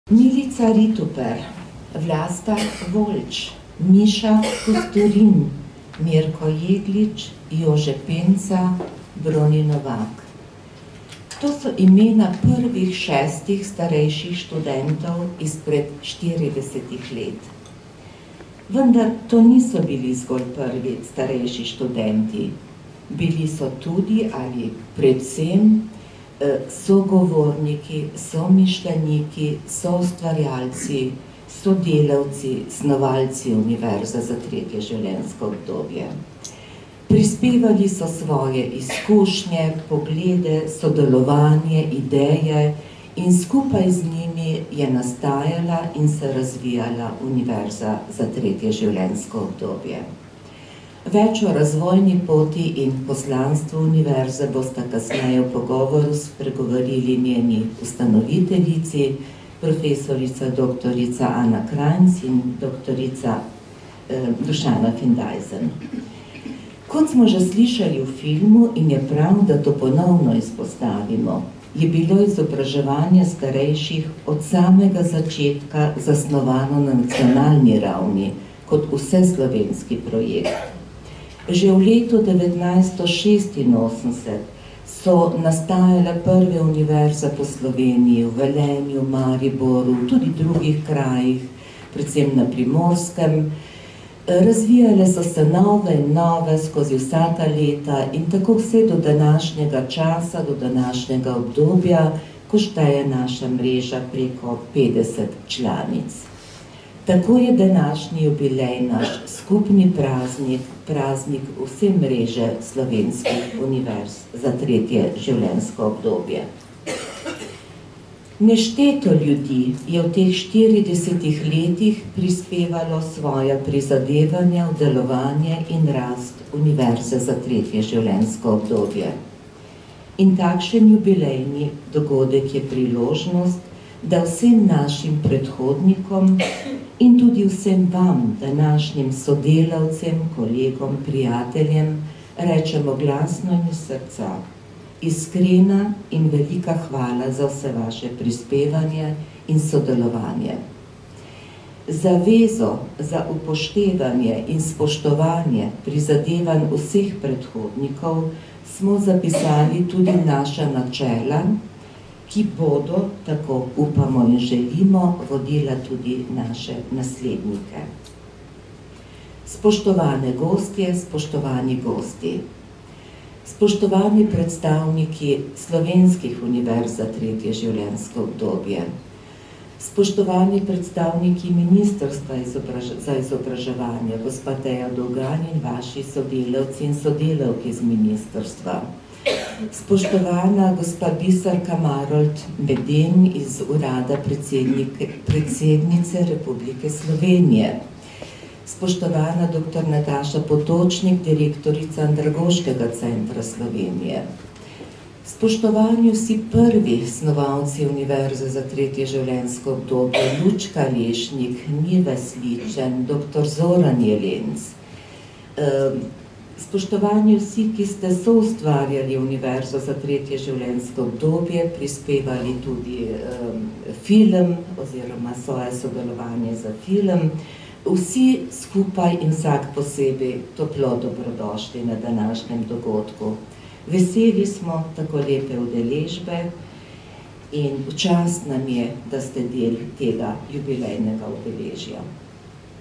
20240410 LJ NJENIH 40 LET ALIJANA SANTEJ SUTZO
Ljubljana, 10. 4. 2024 - V teh 40 letih je Slovenska univerza za tretje življenjsko obdobje (SUTŽO) po besedah Alijane Šantej prehodila "dolgo pot, polno nenehnih in vztrajnih prizadevanj za razvoj izobraževanja starejših, dolgo pot številnih dosežkov, pa tudi ovir in porazov".